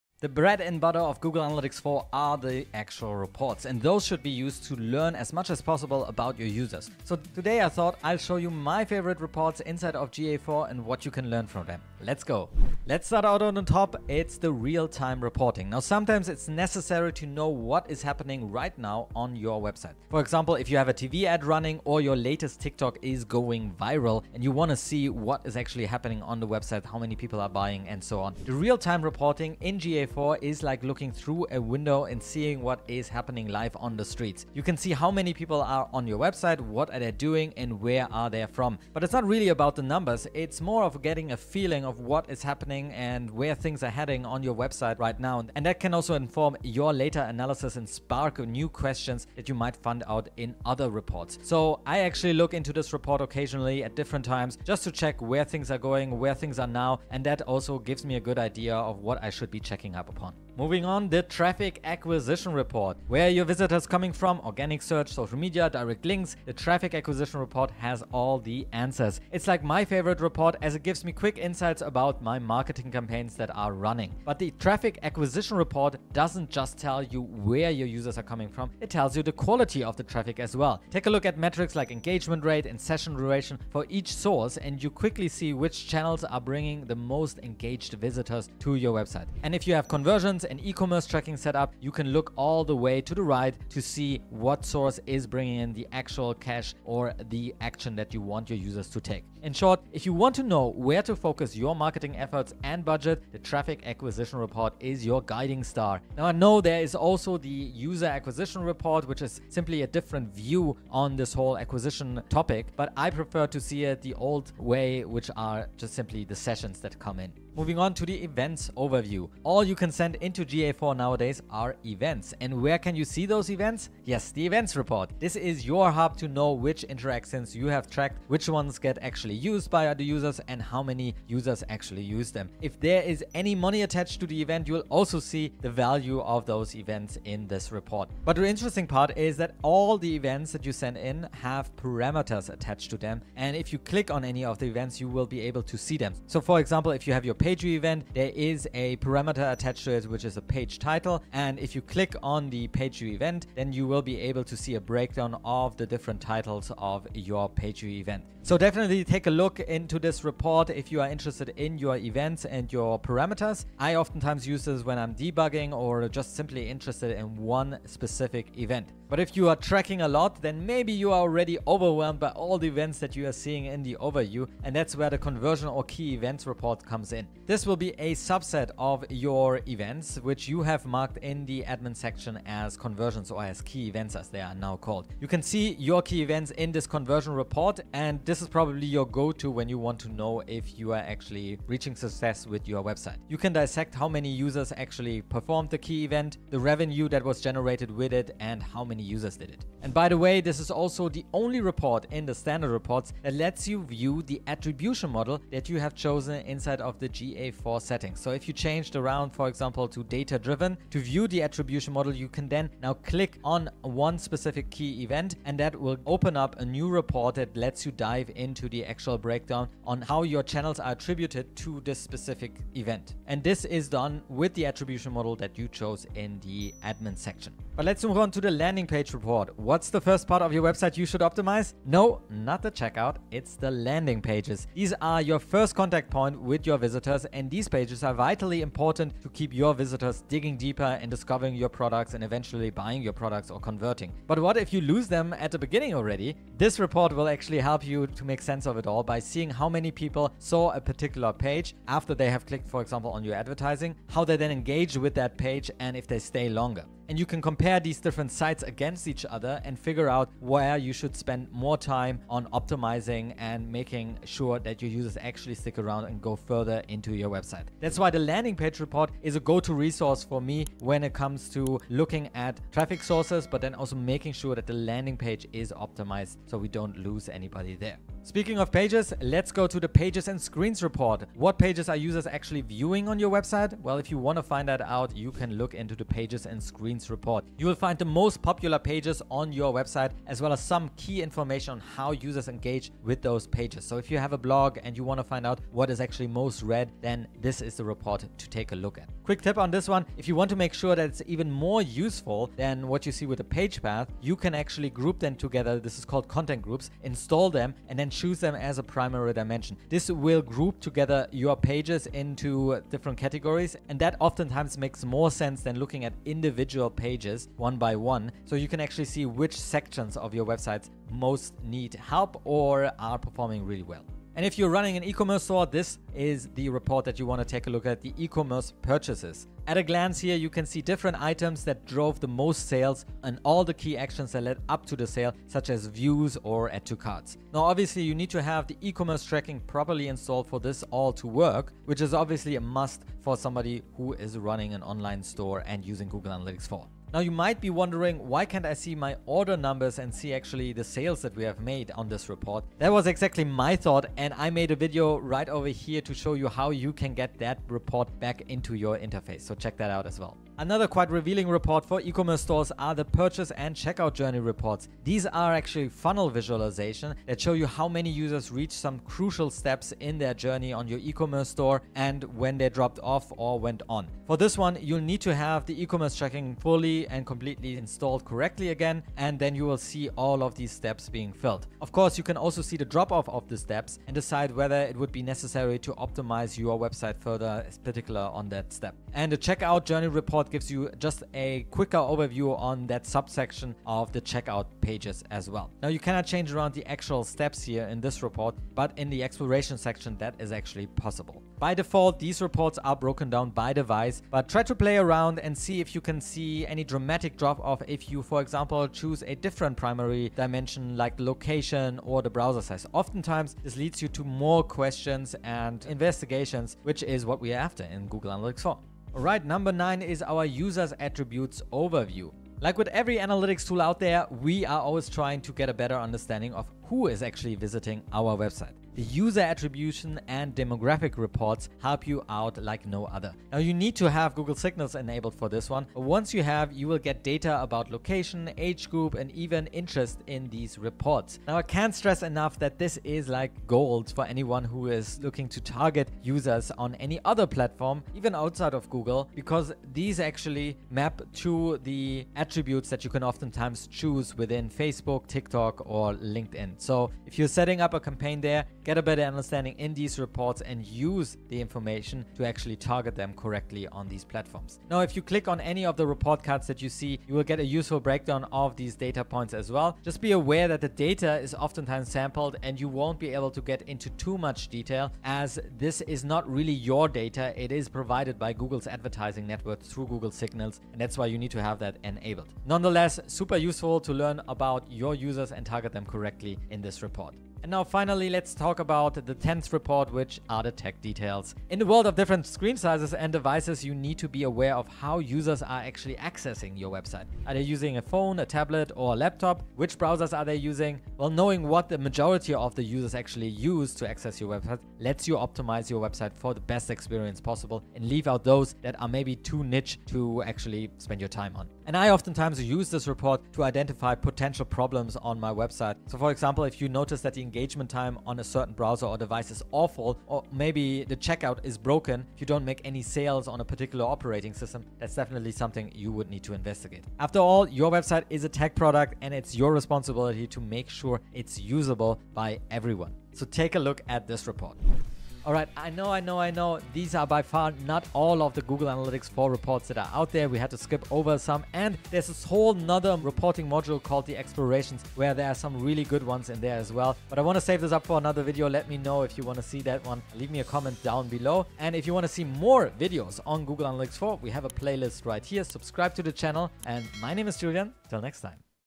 10 Must-Know GA4 Reports
Find out the top 10 Google Analytics 4 (GA4) reports that are crucial for your business's online success. In this video, we'll guide you through each of these reports, explaining why they are essential and how they can provide valuable insights into your website's effectiveness.

👍  FOLLOW US 👍

OUTLINE

00:00 - Introduction
00:12 - 1. Real-time report
01:00 - 2. Traffic Acquisition report
01:57 - 3. Events Overview report
02:54 - 4. Conversion/Key reports
03:47 - 5. Landing Page report
04:38 - 6. Pages and Screens report
05:27 - 7. E-commerce Purchases report
06:02 - 8. Purchase & Checkout Journey reports
06:59 - 9. User Attributes reports
08:09 - 10. Tech Details report
08:55 - Summary